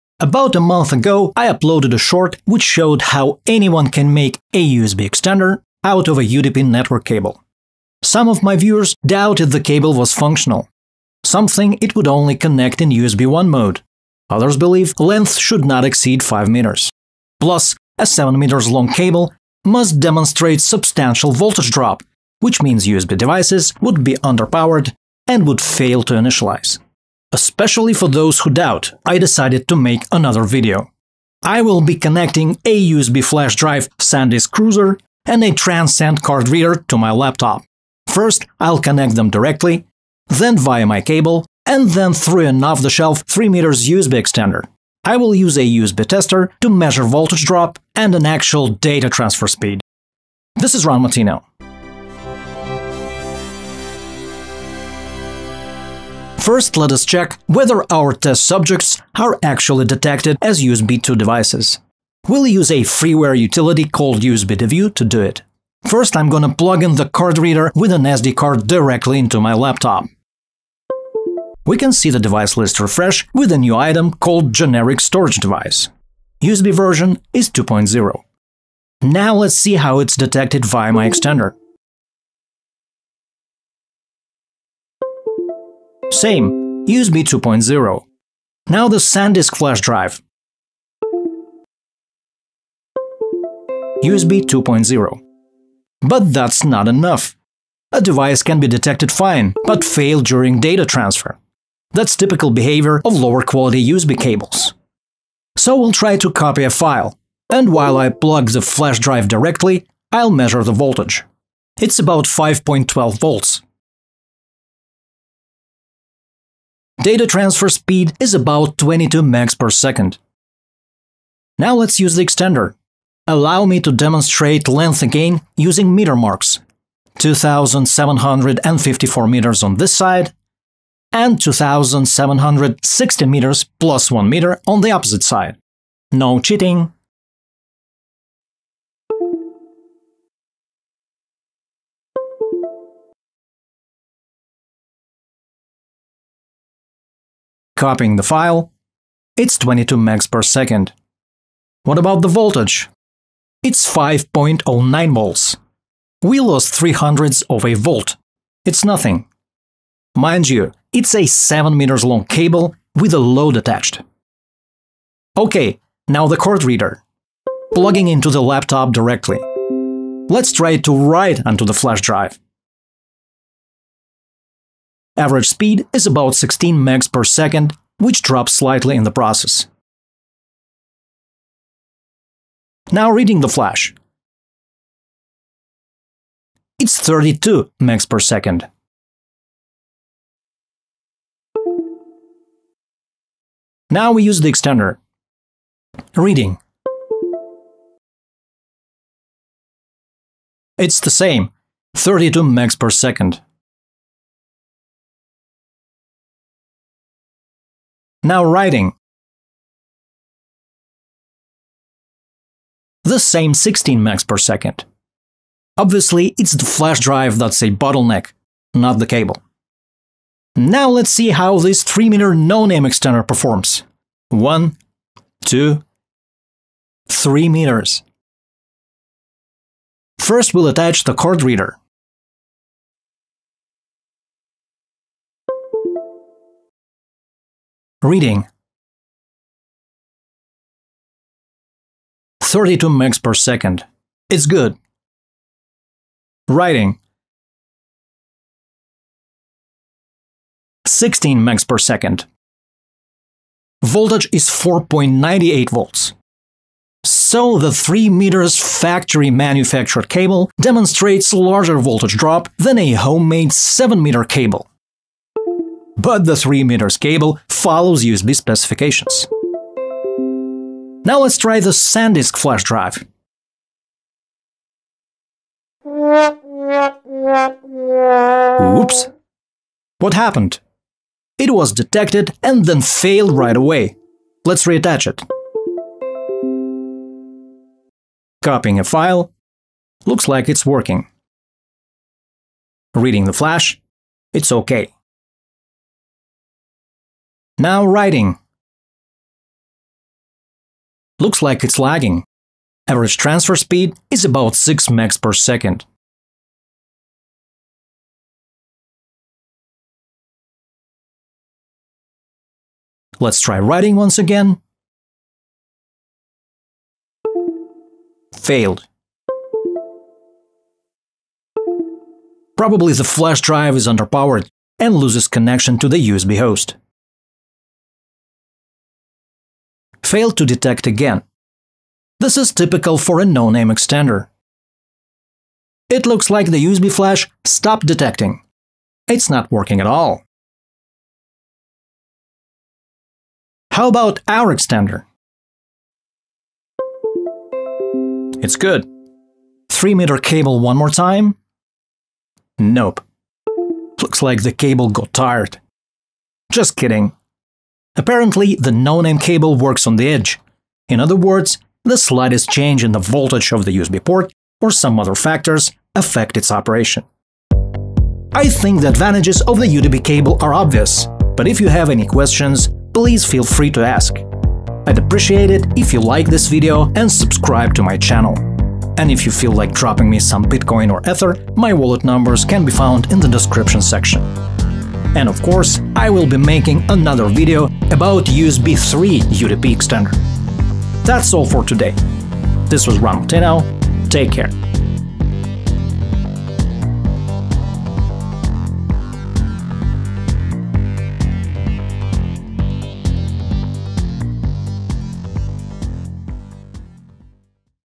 USB over UTP cable: Testing Voltage Drop and Transfer Speed.
BTC:384FUkevJsceKXQFnUpKtdRiNAHtRTn7SD
ETH: 0x20ac0fc9e6c1f1d0e15f20e9fb09fdadd1f2f5cd